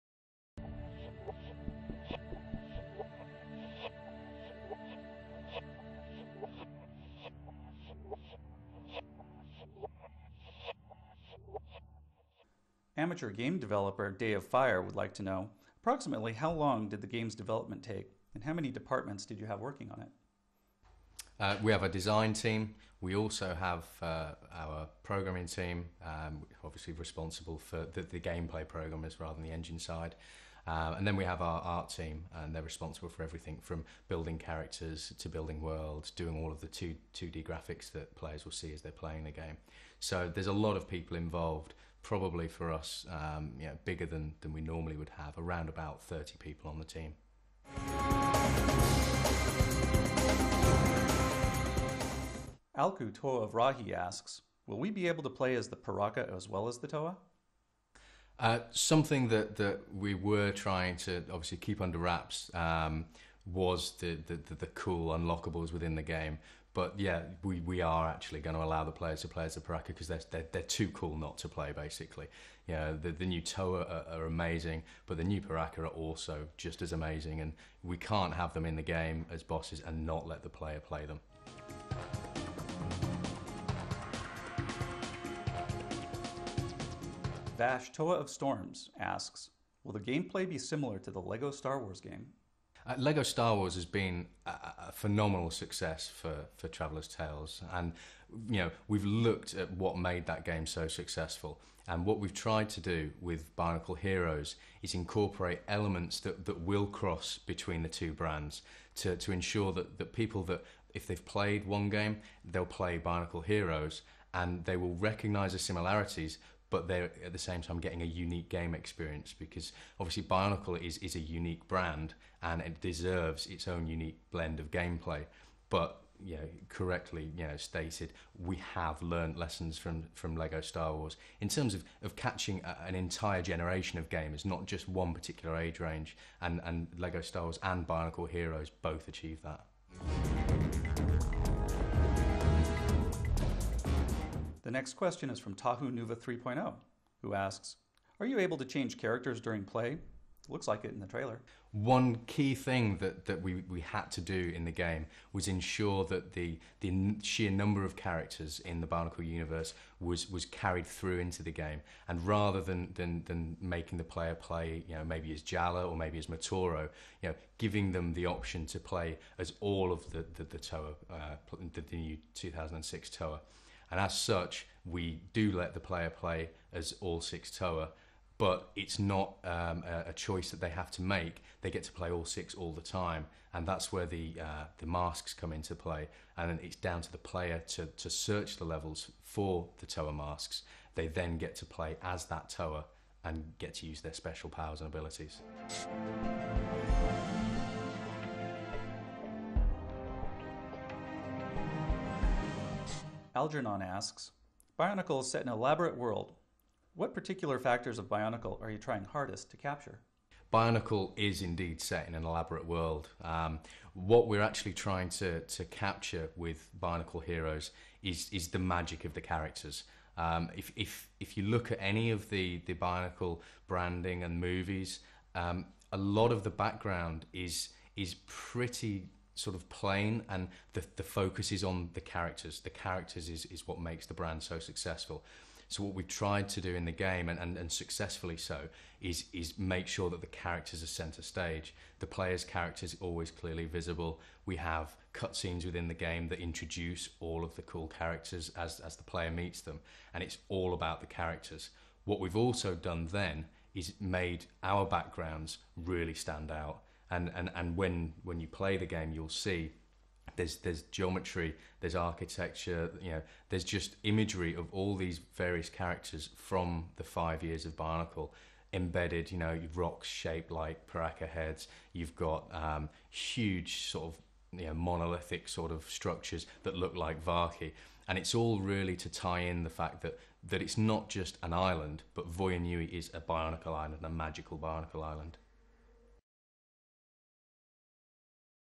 BIONICLE Heroes - BZPower Q&A (Part 2)
Q&A with the design lead of BIONICLE Heroes at TT Games, Arthur Parsons conducted by Kelly McKiernan of BZPower.com in 2006